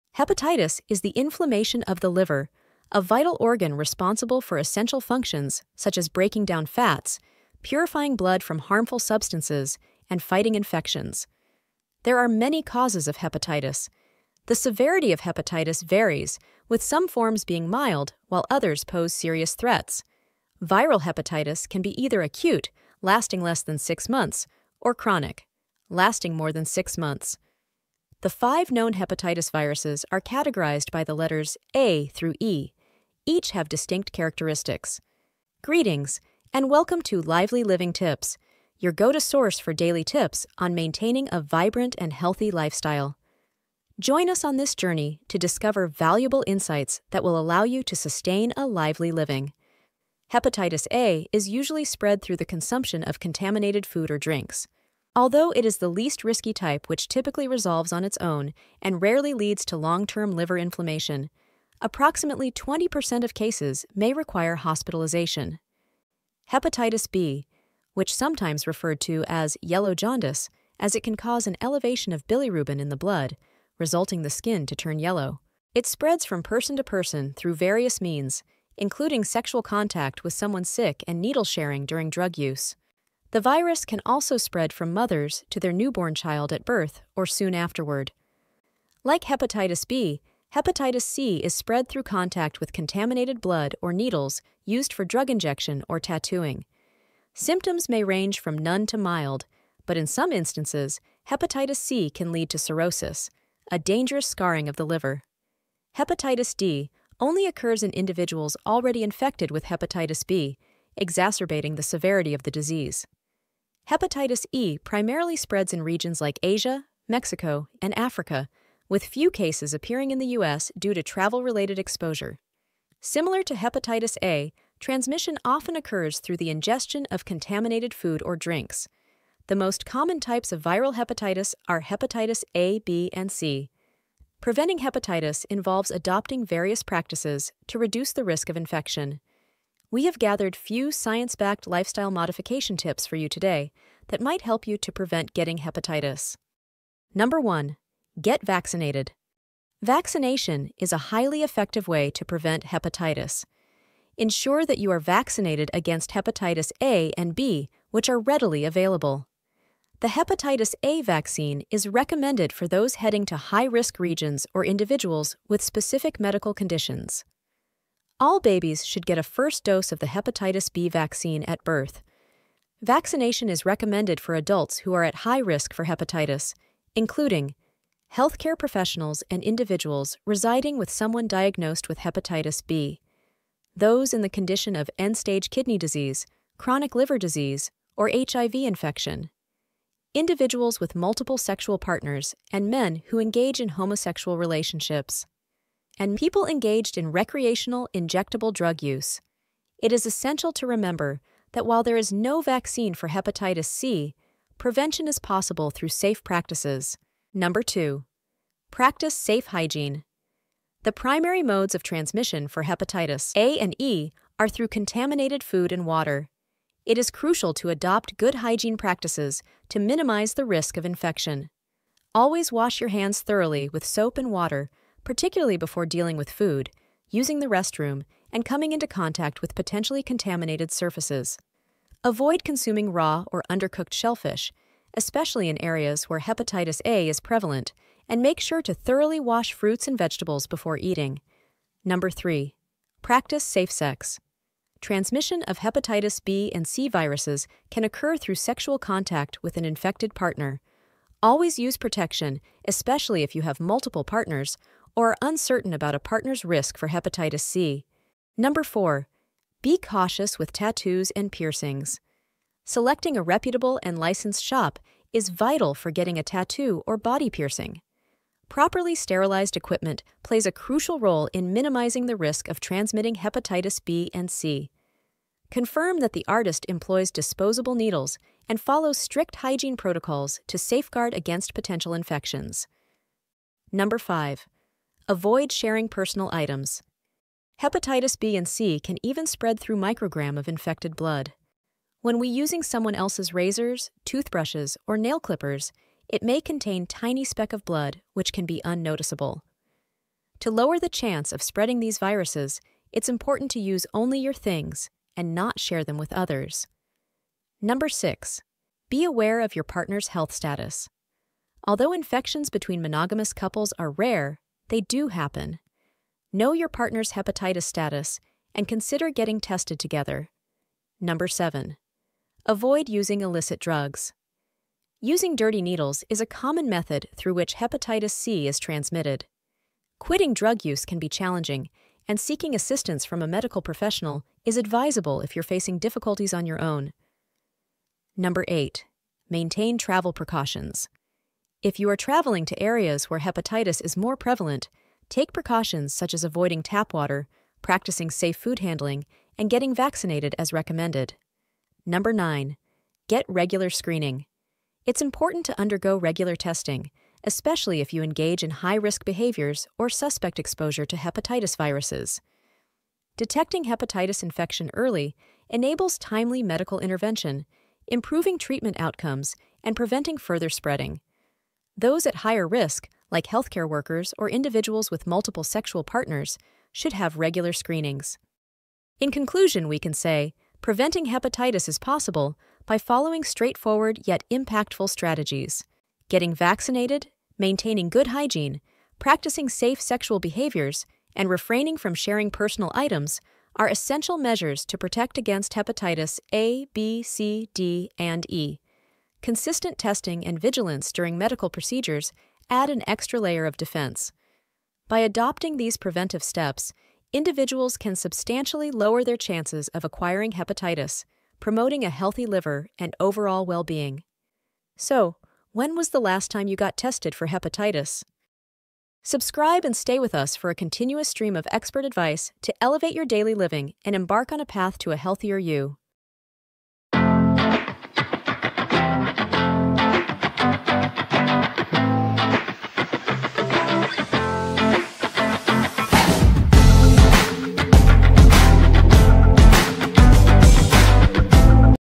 Tips to Prevent HEPATITIS | Hepatitis A, B, C | Jaundice
🩺 Worried about the potential risk of Hepatitis and eager to take proactive steps to prevent it? You're not alone! In today's video, we're delving into essential science-backed tips that can significantly lower your risk of Hepatitis and contribute to overall well-being.

🌿 From incorporating simple lifestyle changes to making thoughtful dietary adjustments, we've compiled a comprehensive guide based on research outcomes to empower you in your journey to prevent Hepatitis.

In this video, we're breaking new ground by utilizing state-of-the-art artificial intelligence technology for the narration.